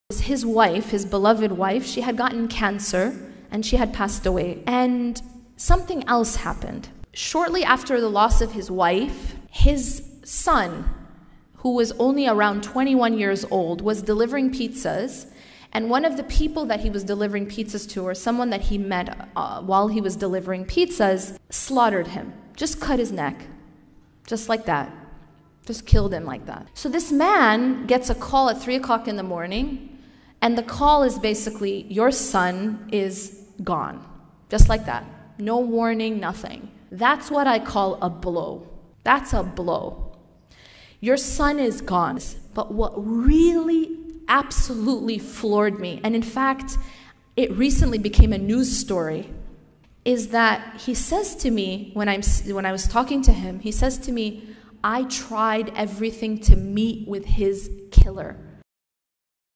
A Story That Made Yasmin Mogahid Cry | The Power of Forgiveness | Full Video Click ▶︎
When you forgive someone who wronged you, you will get something priceless in return, His forgiveness. And we are all in desperate need of forgiveness–from God and from the people.Forgive. So that you too may be forgiven. Be grateful for every single person who was part of your story. The ones that hurt you. The ones that helped you. Because they all taught you. (forgiveness islamicvideo islamiclectures islamicstatus)

Video source: Yasmin Mogahid